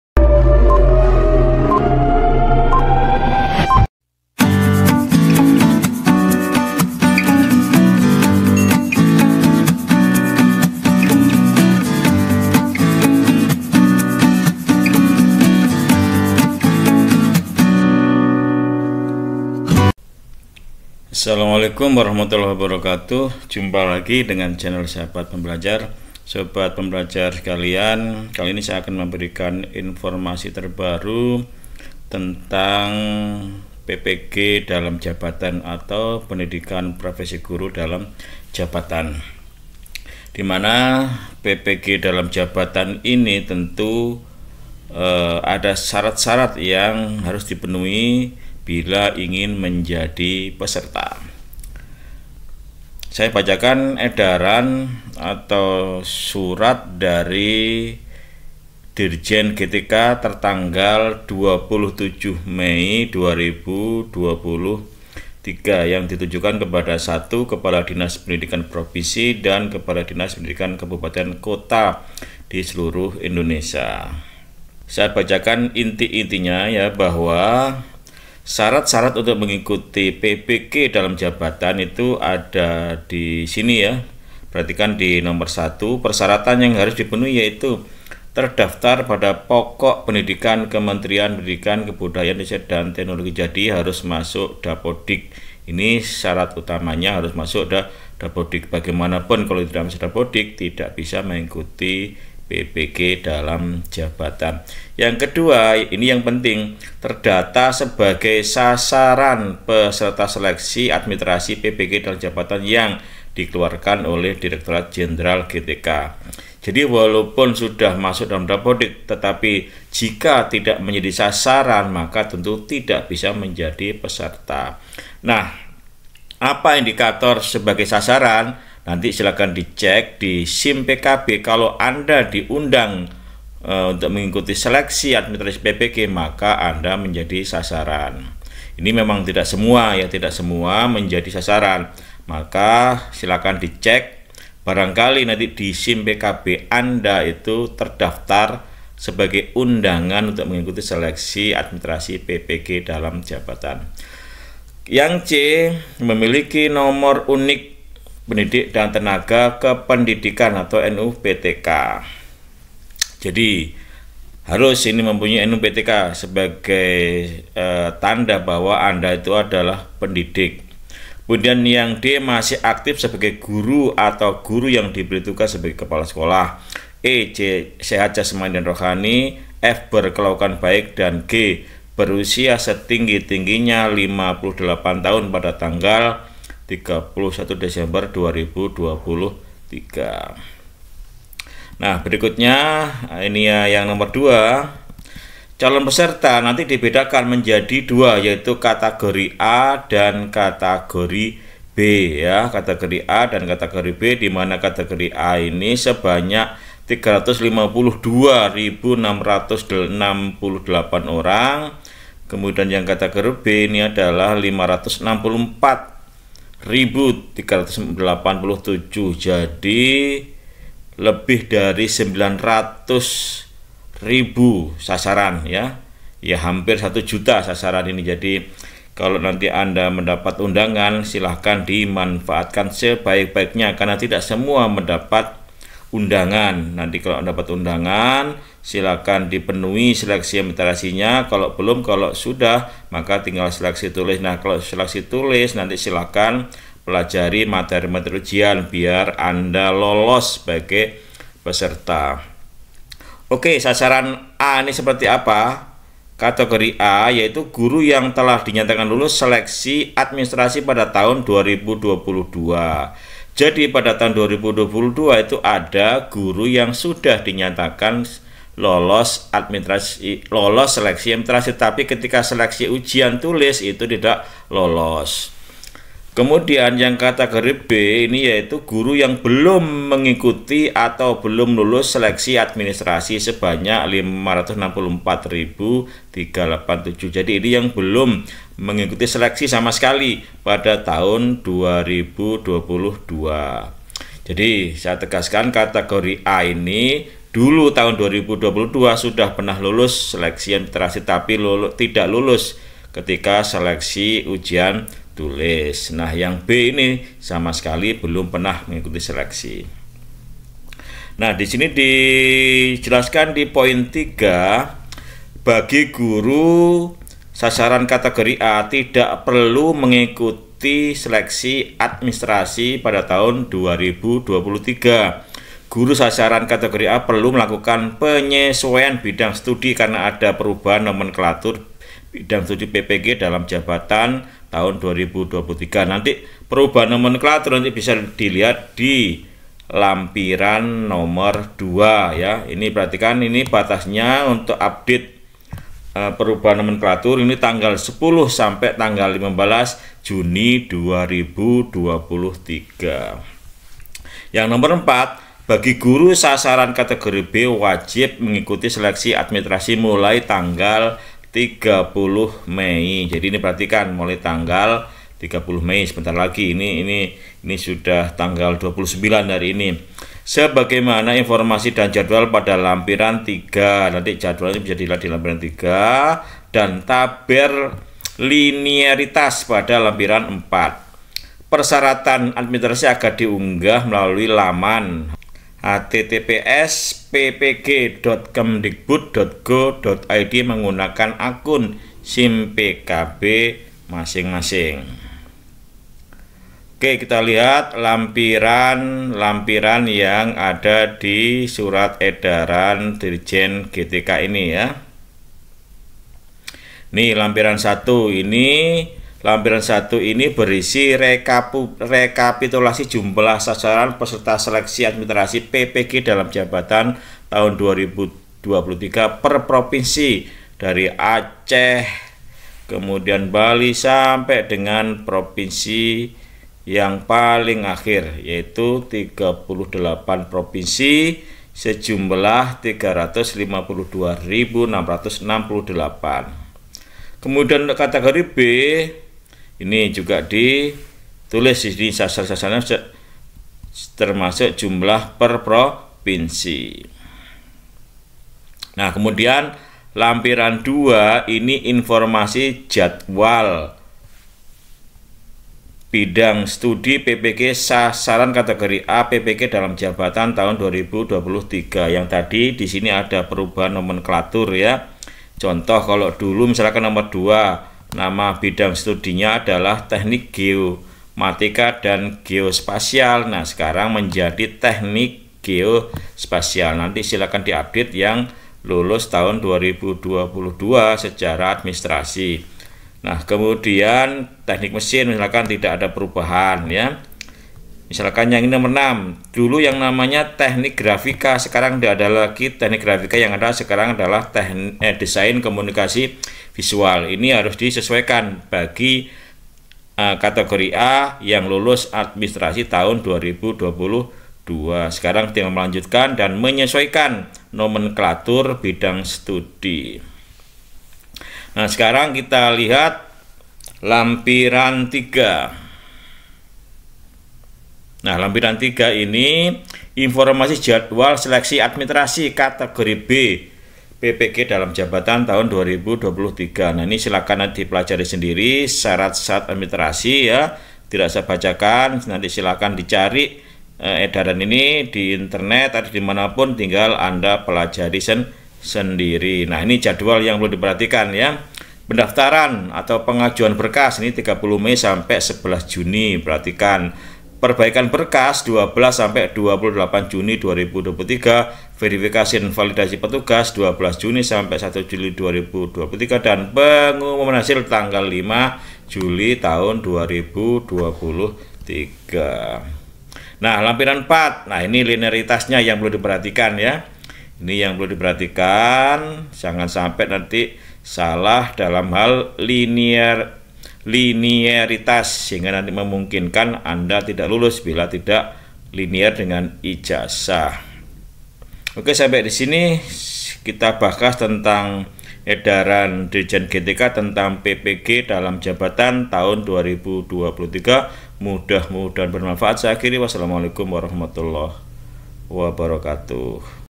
PENTING !!! GURU JANGAN SAMPAI KETINGGALAN.SEGERA BUKA SIM PKB ANDA. PPG DALJAB 2023 SEGERA DIBUKA
PPG Daljab 2023 segera dibuka. Penting bagi guru atau saudar guru, adiknya guru, yang belum sertifikasi untuk mengetahui ini